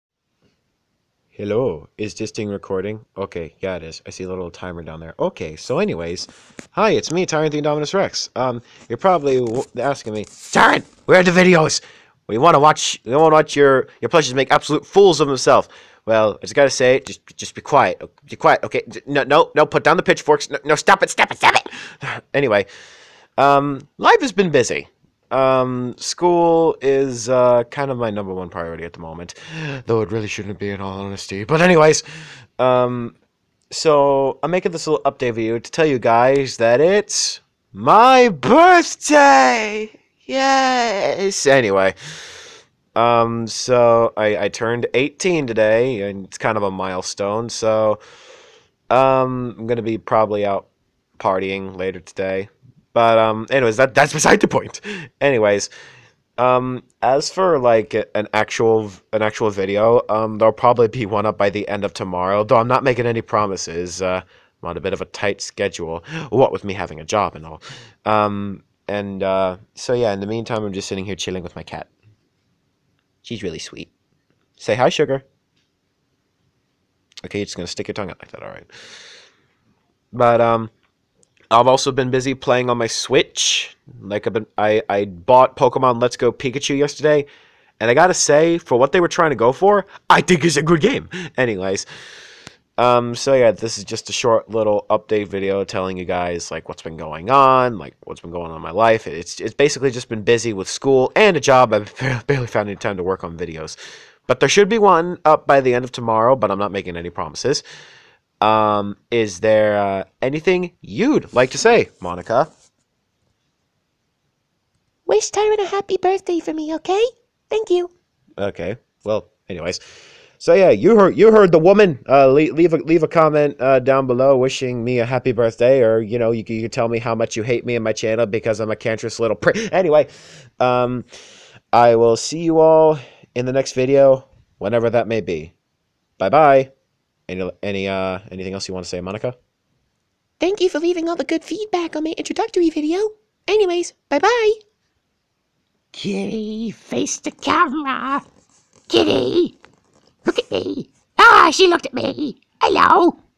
Birthday update video thing....Yaaaaaayyy :D
That's right, as of today, I'm a legal adult :D Anyways, this is nothing more than a quick update video, just wanted to inform you guys of what was going on in me life to keep me from uploading, anyways, see ya in da next video, bye